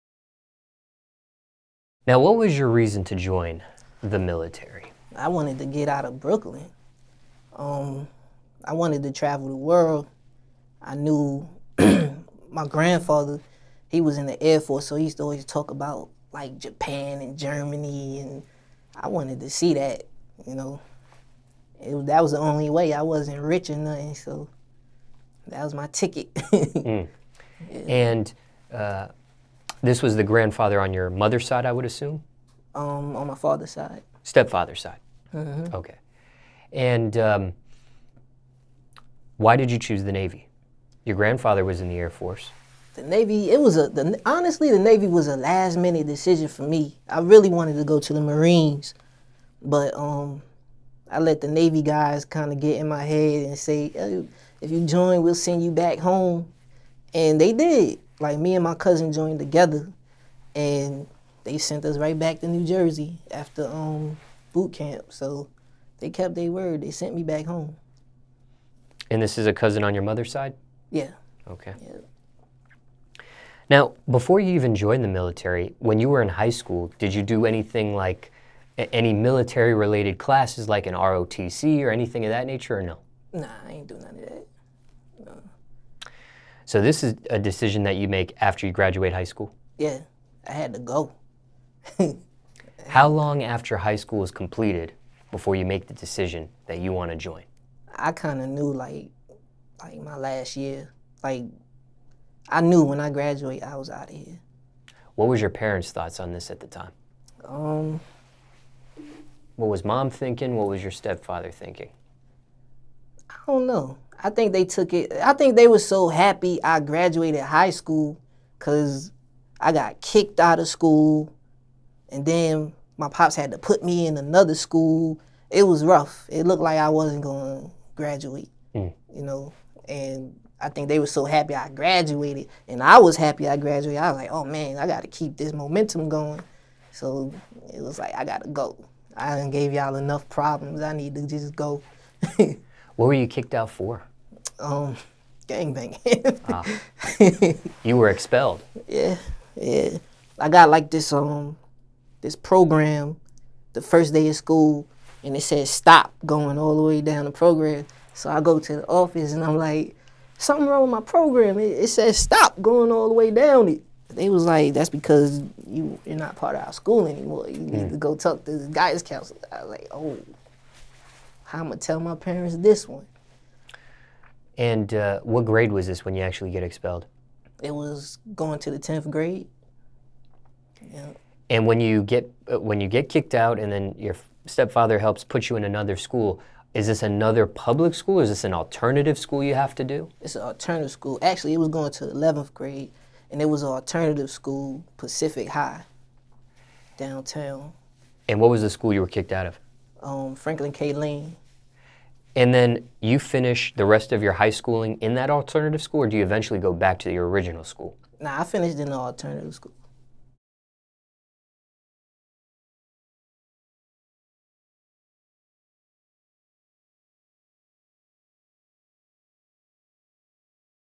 Fase Expelled For Gang Banging in High School, Why Joined Navy After
Brooklyn East New York born/Atlanta residing recording artist, Fase, sits down with DJ Smallz and reflects on his upbringing, sharing insight on his schooling and family. This is part 3 from the full interview.